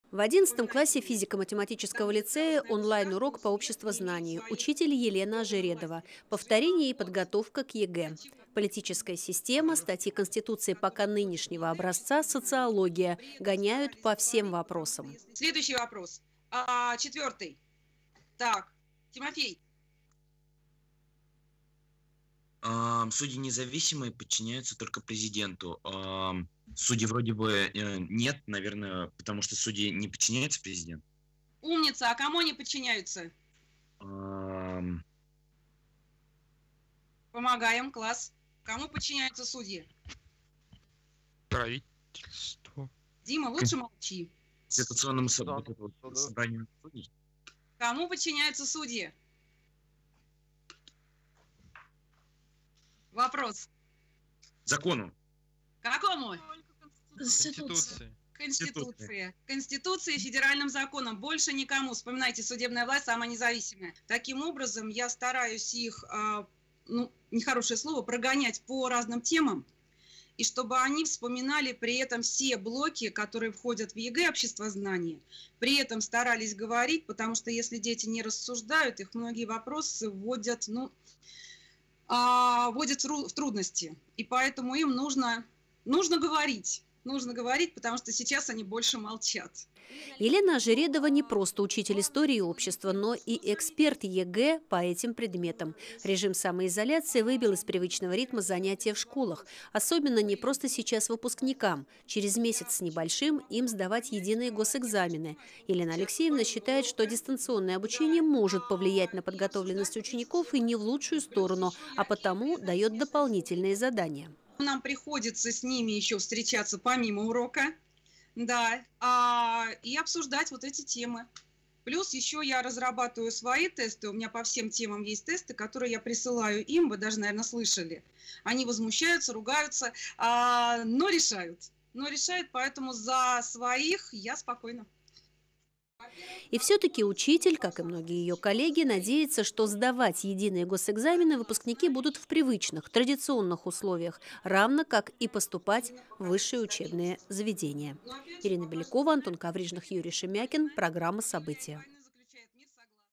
В физматлицее готовятся к ЕГЭ онлайн, но надеются, что сдавать будут в реале | Сергиев Посад
В 11 классе физико-математического лицея идёт онлайн-урок по обществознанию. Учитель Елена Ожередова повторяет с учениками темы и готовит их к ЕГЭ.

Политическая система, статьи действующей Конституции, социология – выпускников прогоняют по всем вопросам. Не сразу, но лицеисты находят ответ на вопрос, кому подчиняются судьи – Основному и федеральным законам, больше никому.

Таким образом я их прогоняю по разным темам, чтобы они вспоминали все блоки, входящие в ЕГЭ. Нужно, чтобы они говорили, потому что, если не рассуждать, многие вопросы могут вызвать трудности. Нужно говорить, хотя сейчас они больше молчат, – Елена Ожередова, учитель истории и обществознания физико-математического лицея

Елена Ожередова не просто учитель истории и обществознания, но и эксперт ЕГЭ по этим предметам. Режим самоизоляции выбил из привычного ритма занятия в школах. Особенно не просто сейчас выпускникам. Через месяц с небольшим им сдавать Единые госэкзамены. Елена Алексеевна считает, что дистанционное обучение может повлиять на подготовленность учеников, и не в лучшую сторону, а потому даёт дополнительные задания.

Нам приходится с ними встречаться помимо урока и обсуждать эти темы. У меня по всем темам есть свои тесты, которые я отсылаю им. Они возмущаются, но решают, поэтому за своих я спокойна, – Елена Ожередова

И всё-таки учитель, как и многие её коллеги, надеется, что сдавать Единые госэкзамены выпускники будут в привычных условиях, равно как и поступать в высшие учебные заведения.